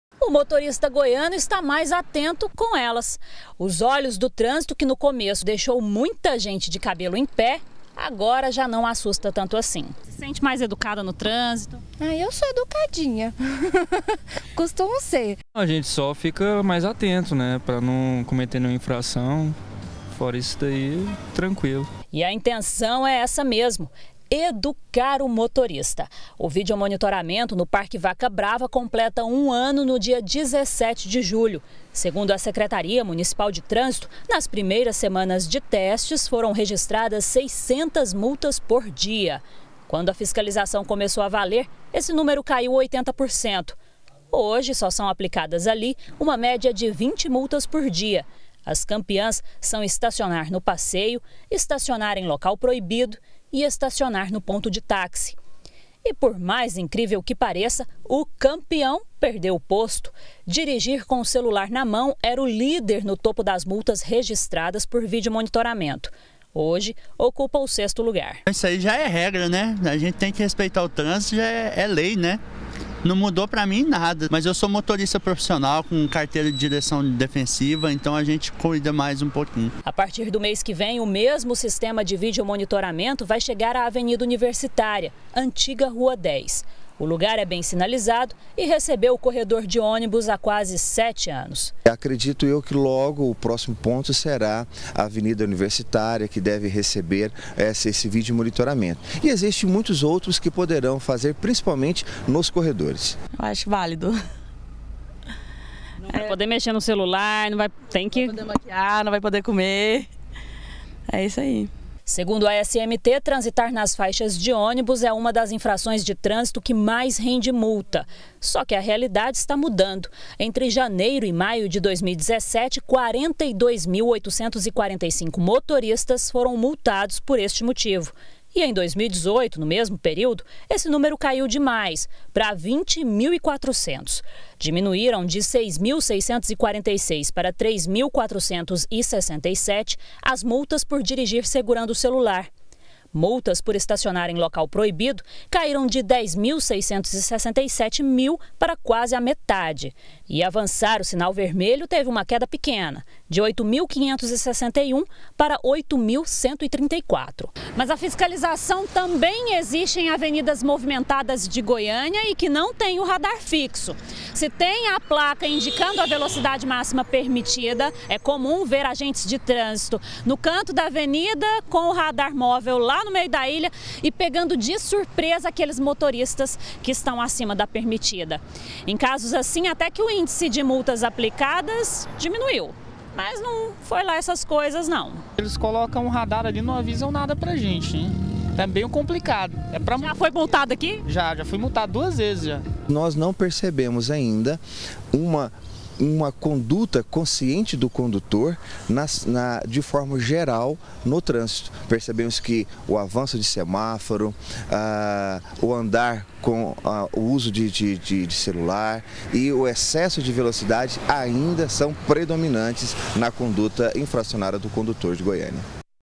BG - Número de multas cai em Goiânia - 22-06-2018
O número de multas de trânsito aplicadas aqui na capital diminuiu. Para a SMT, esta redução é resultado da educação do motorista e do rigor da fiscalização.